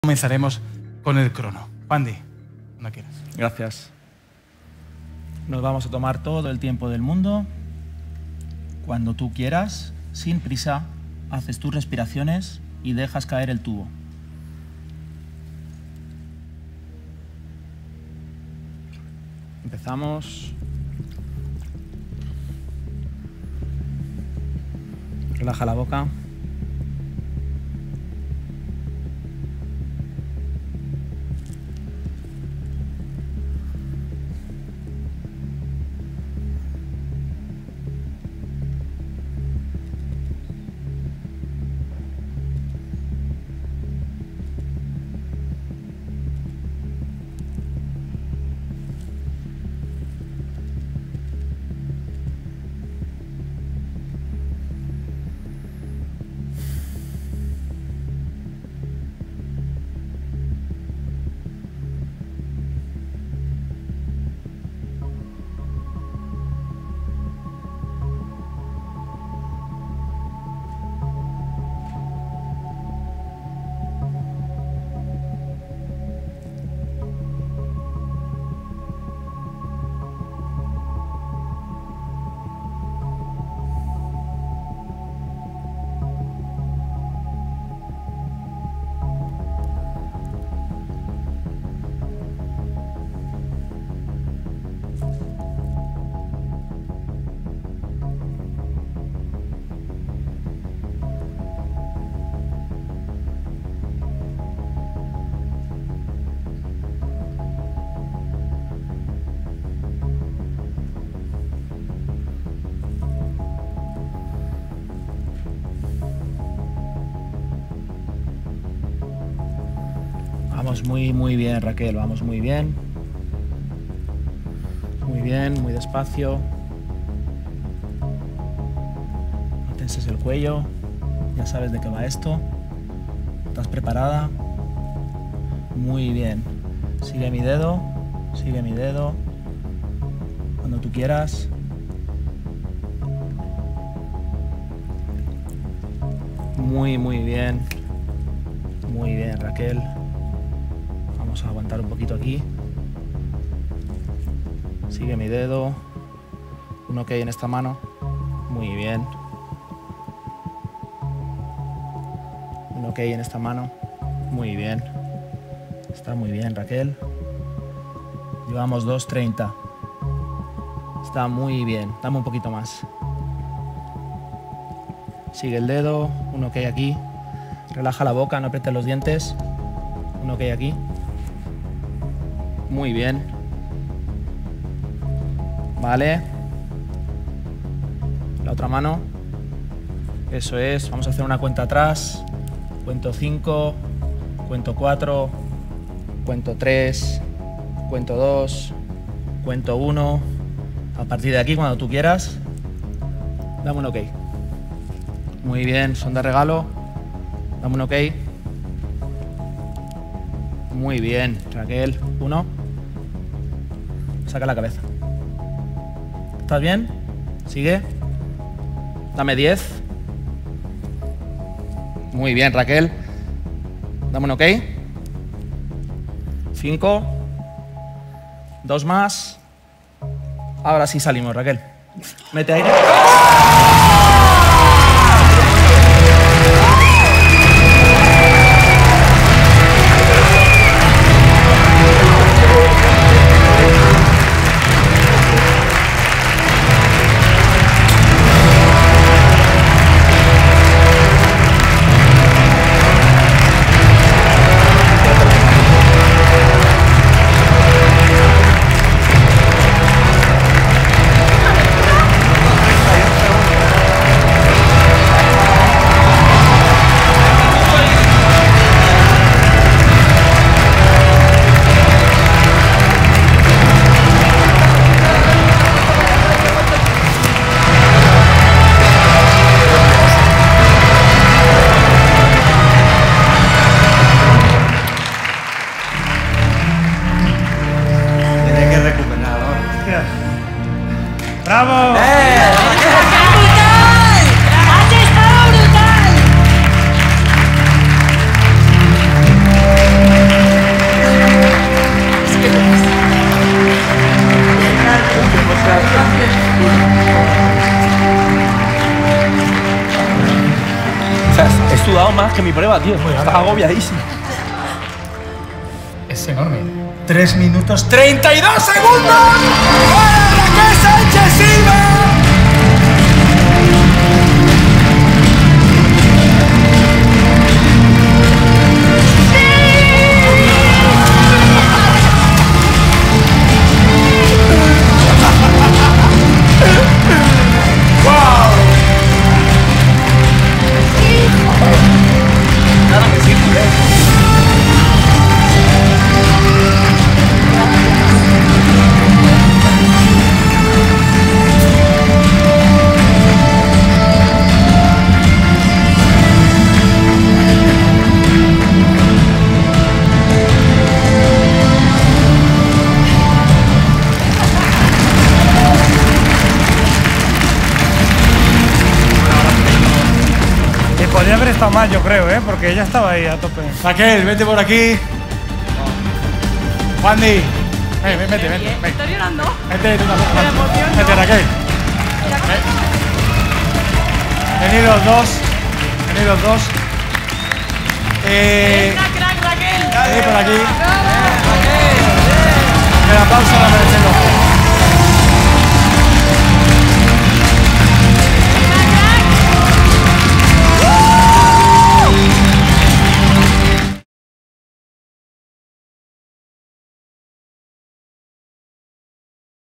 Raquel Sánchez-Silva nos emociona con su desafío en apnea
La presentadora hace una increíble prueba de apnea demostrando su gran capacidad de superación.